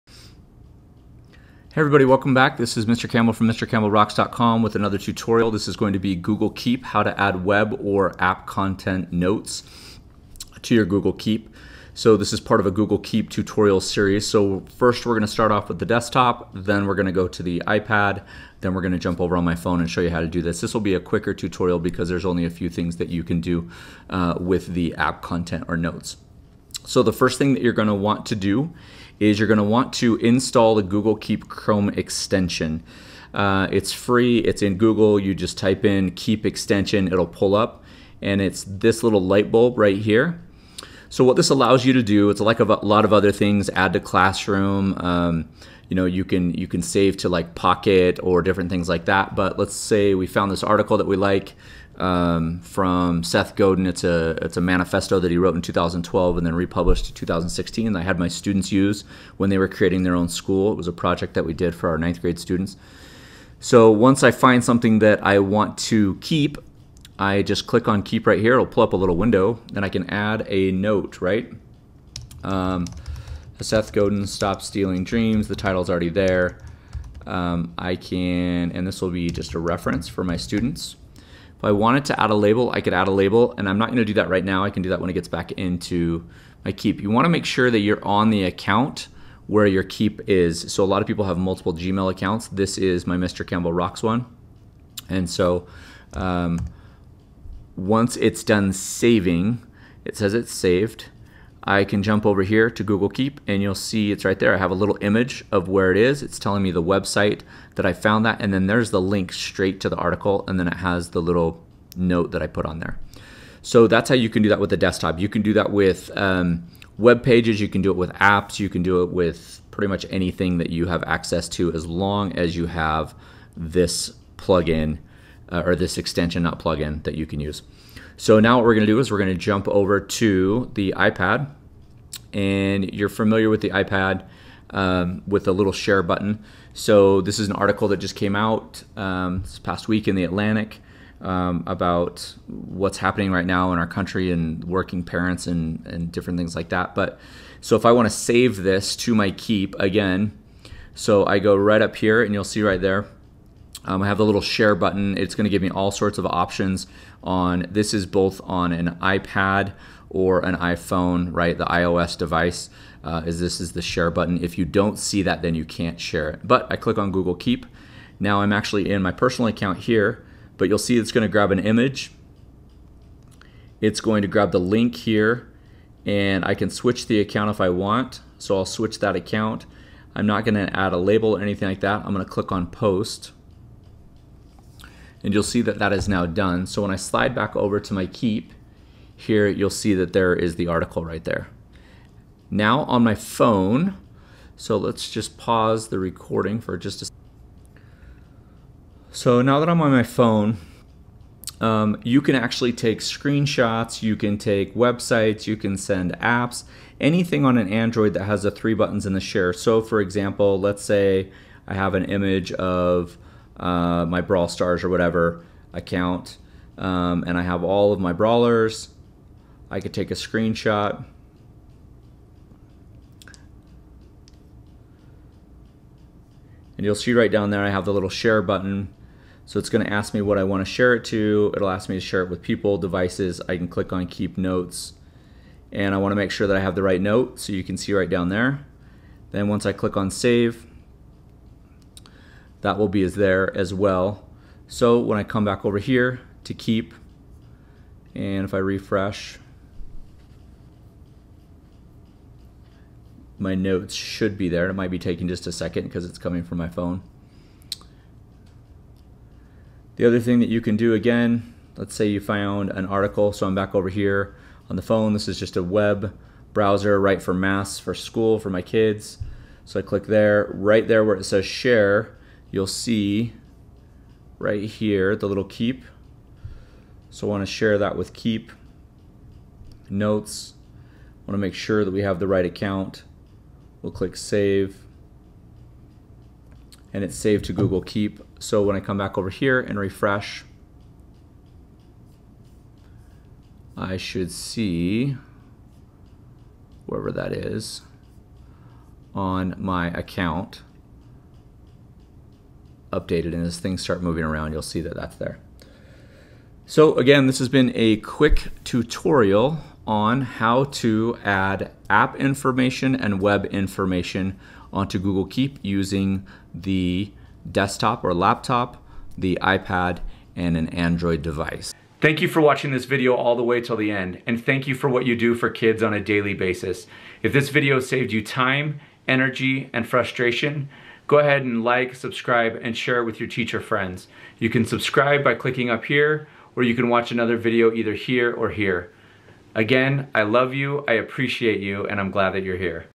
Google Keep: How to Add Screenshots and Websites to Keep
This tutorial explains how to add screenshots and websites to Google Keep on your desktop, IOS device, and Android device.